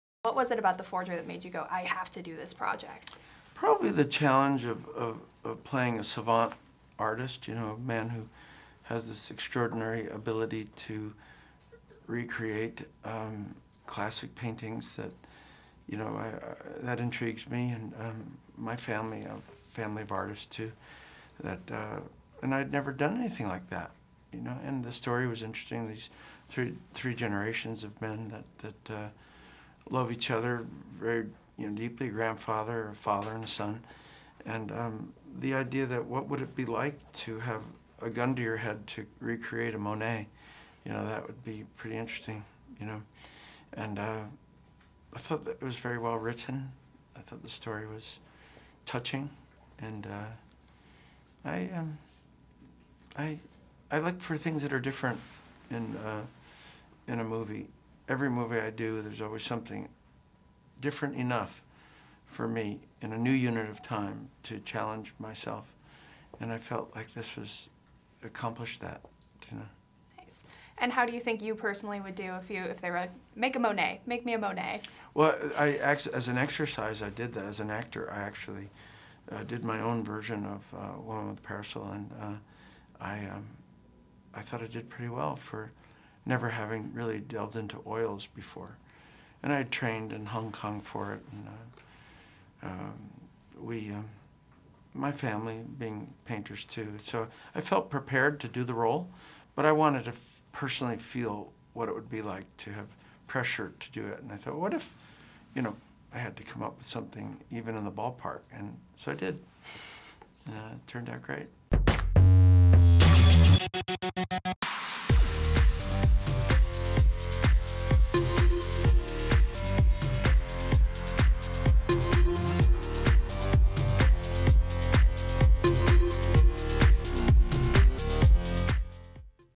John Travolta Painted A Monet For 'The Forger’ | MTV News
John Travolta talks about what attracted him to the project ‘The Forger’ and reveals why he painted his own version of the famous Monet.